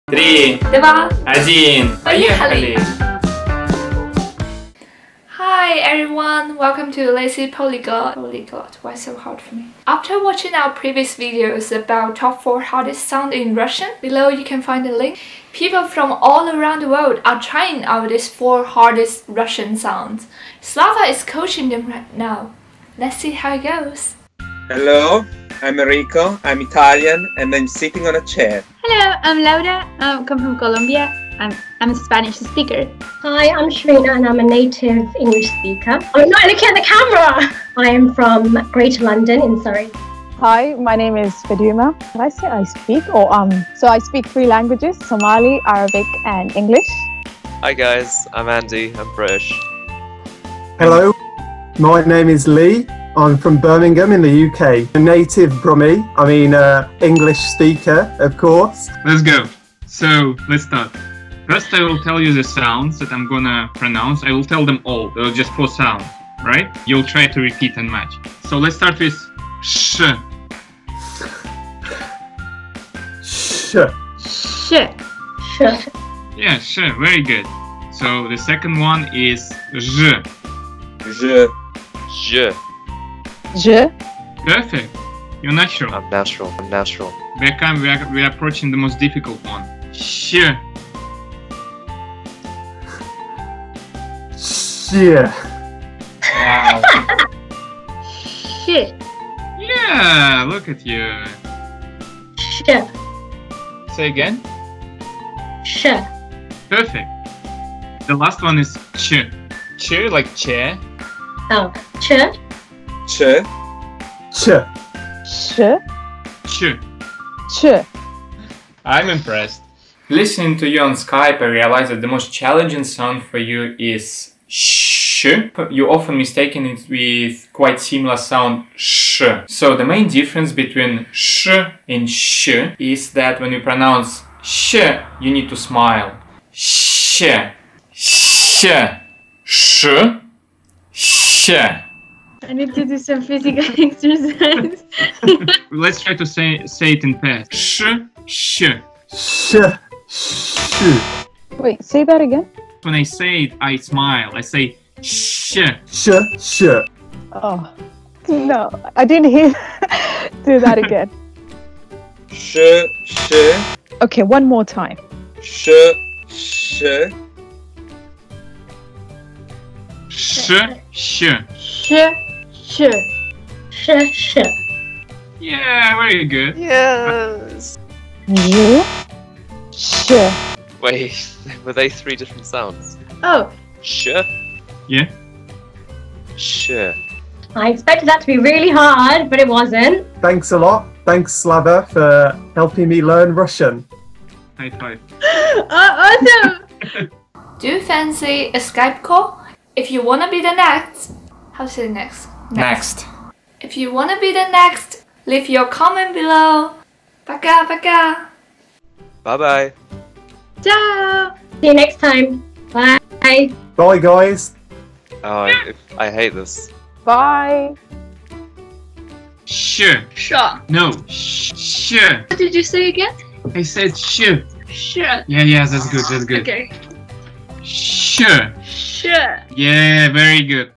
Russian Pronunciation Practice | Ш Ж Щ Ч
Our previous video on the pronunciation of the top 4 Russian sounds gained certain popularity within our friends. They want to pronounce these sounds for themselves.
Unfortunately, the isolation during Covid19 stops us from visiting. Fortunately, we all have a good Internet connection.

In this video, we practice the pronunciation of hushing ш, ж, щ and ч sounds on Zoom with 6 different people (3 English, 1 Italian, 1 Colombian and 1 English/Somali/Arabic speakers).